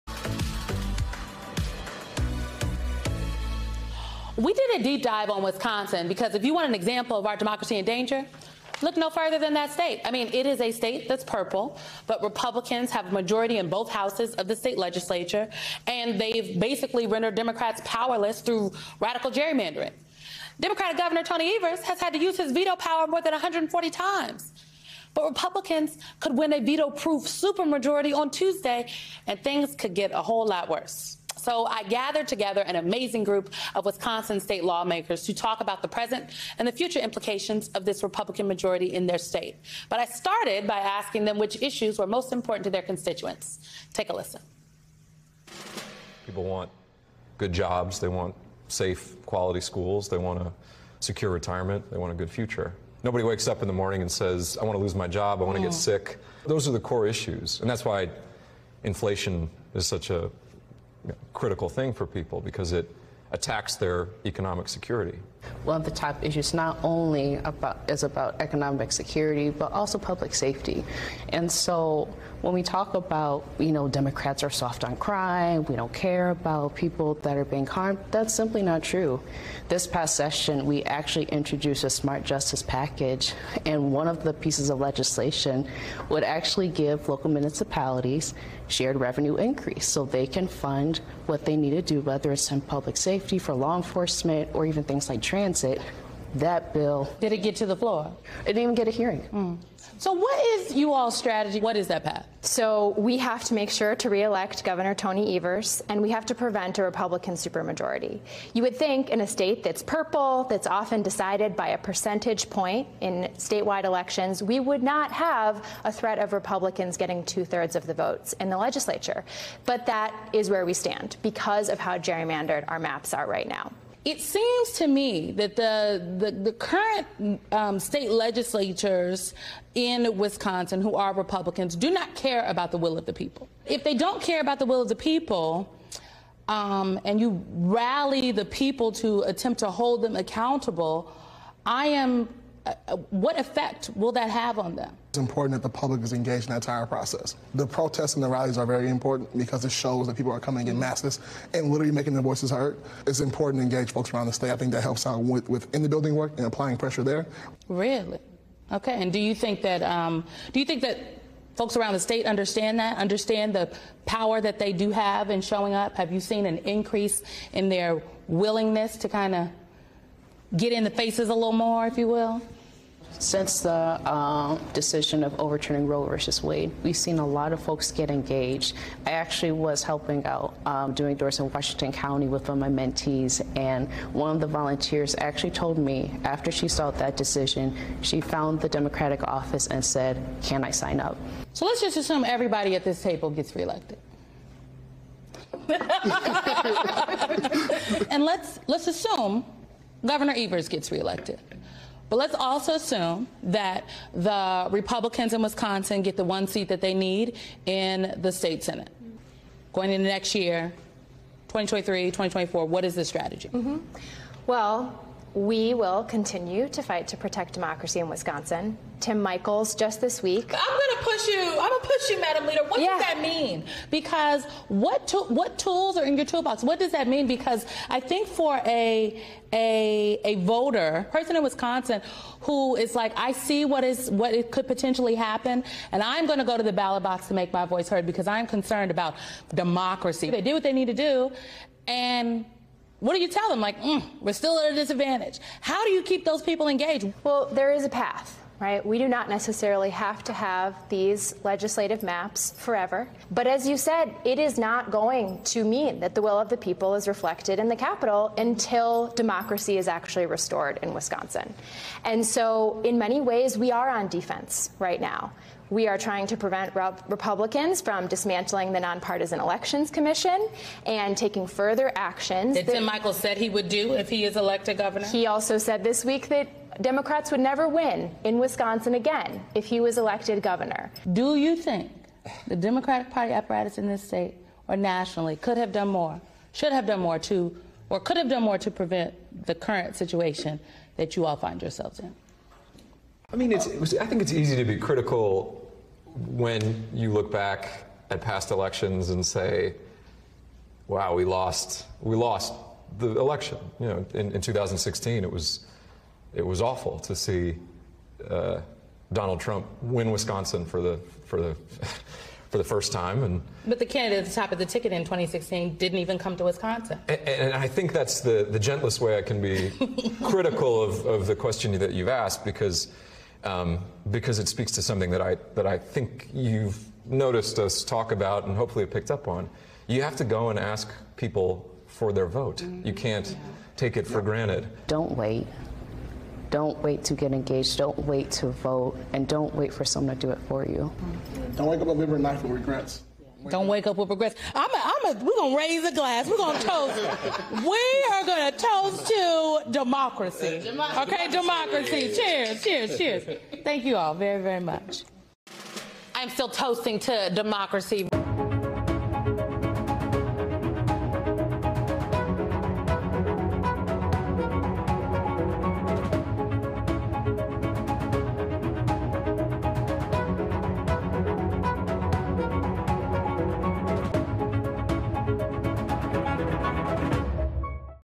Wisconsin State Legislators Weigh In On The Future Of Democracy
A roundtable of Wisconsin state lawmakers discuss the implications of the Republican majority in both houses of the state legislature and how it’s affecting the future of democracy in their state.

The 2022 midterm elections are here, and MSNBC is following the most important races across the United States as Americans cast their votes.